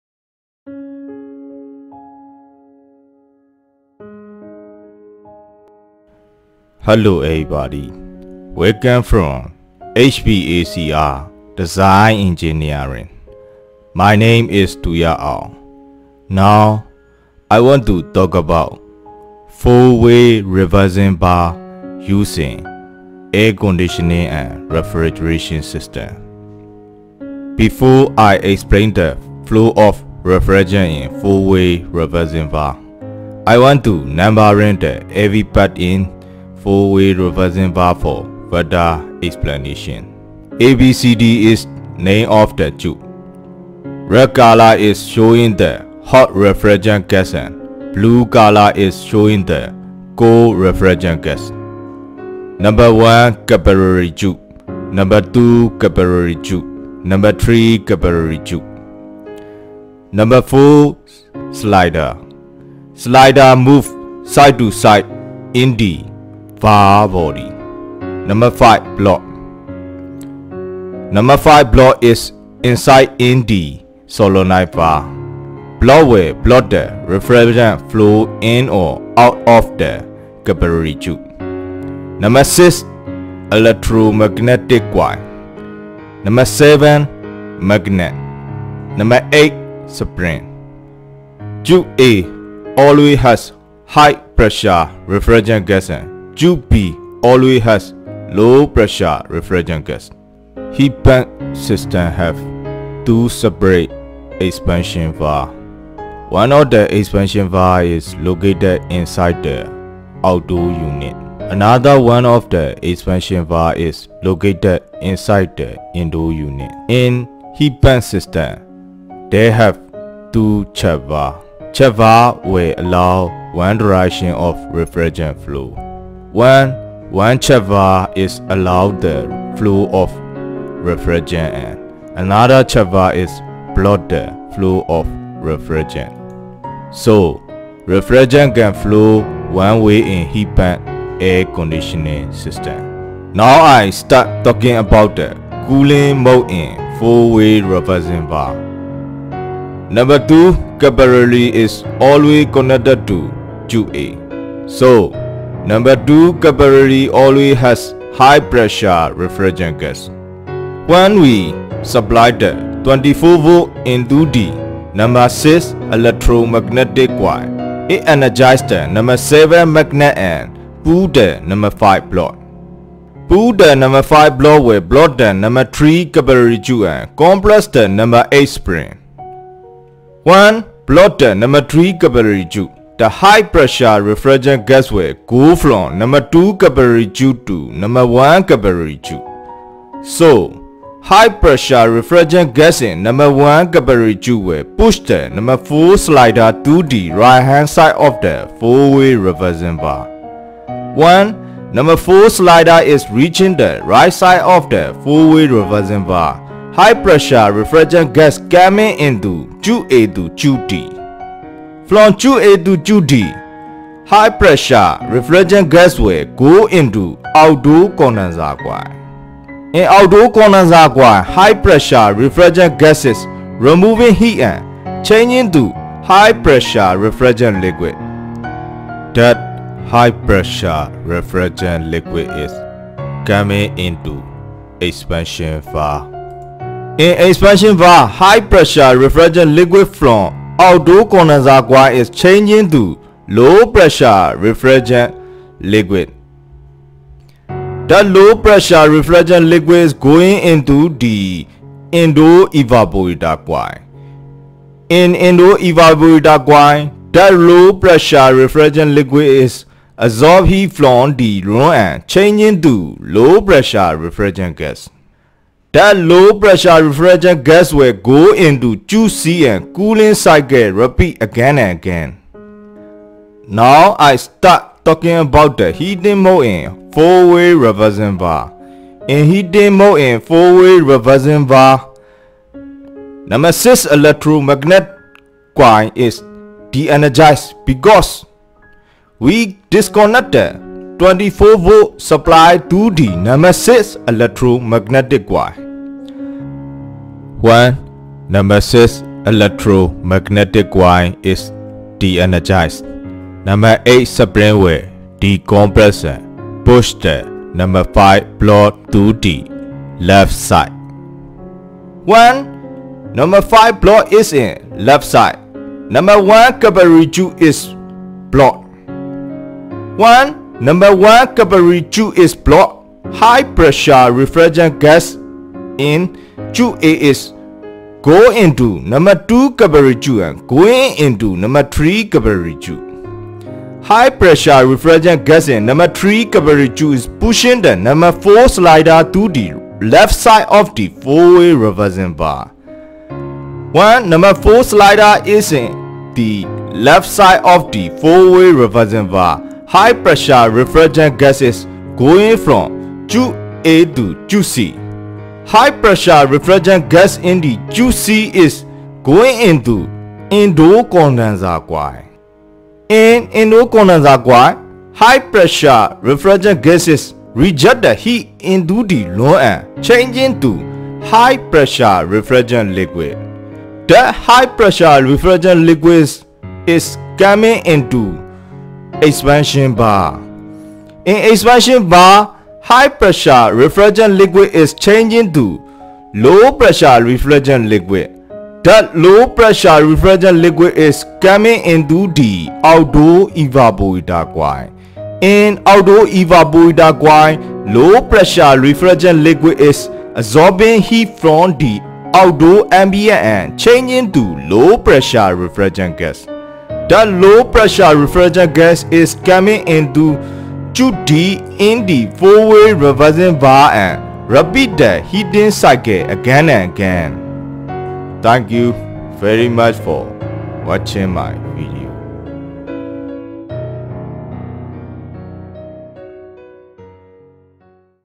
Four Way Reversing Valve Used In Air Conditioning and Refrigeration System
Content:
- Four Way Reversing Valve Location
- Assembly Part For Four Way Reversing Valve
- Cooling Mode In Four Way Reversing Valve